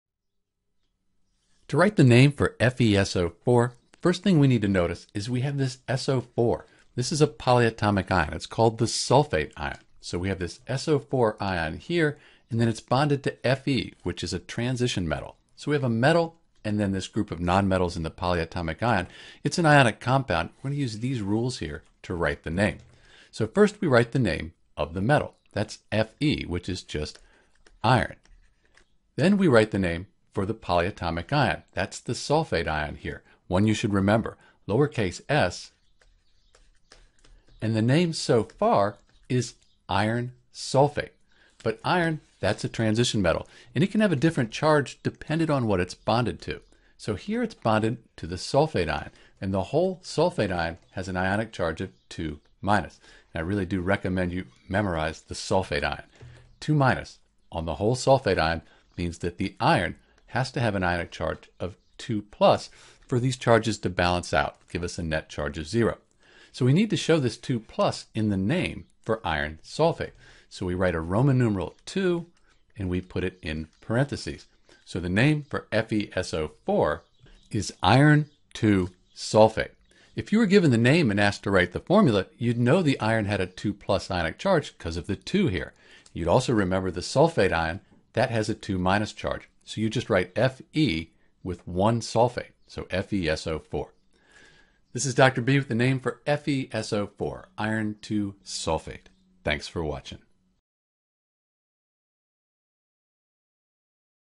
How to Write the Name for FeSO4
In this video we'll write the correct name for FeSO4. To write the name for FeSO4 we’ll use the Periodic Table and follow some simple rules.

Because FeSO4 has a polyatomic ion we’ll also need to use a table of names for common polyatomic ions, in addition to the Periodic Table.

1) Name the metal (the cation) as it appears on the Periodic Table.
2) Find the polyatomic ion on the Common Ion Table and write the name.

For a complete tutorial on naming and formula writing for compounds, like Iron (II) sulfate and more, visit: